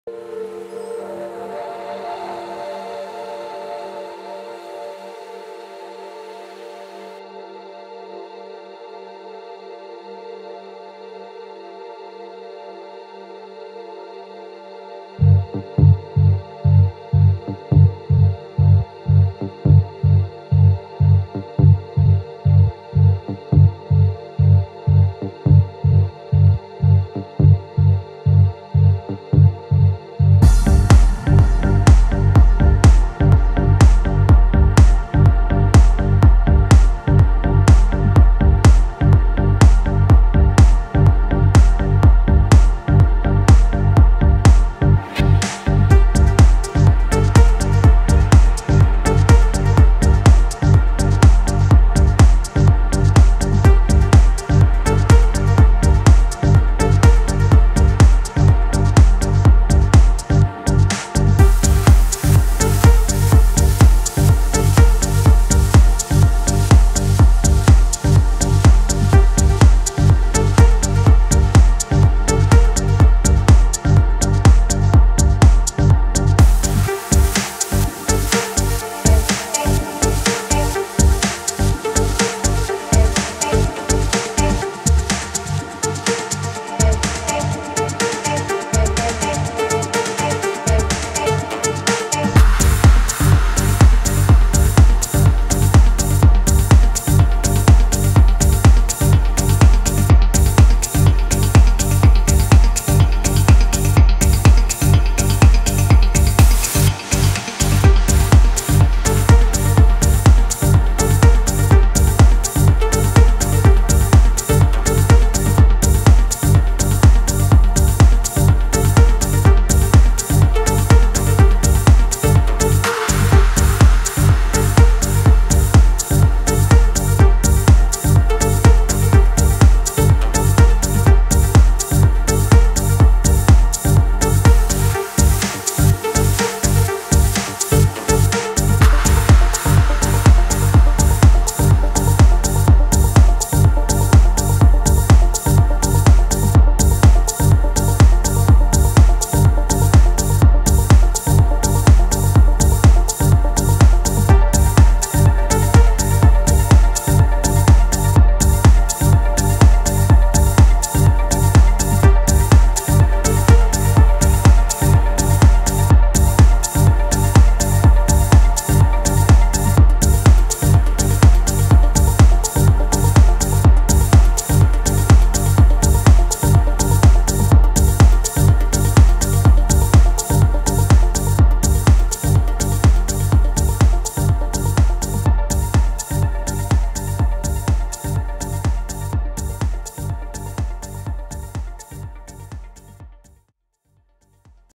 Taranaki Morning3
Remastered - Early morning flight near Mount Taranaki where I live.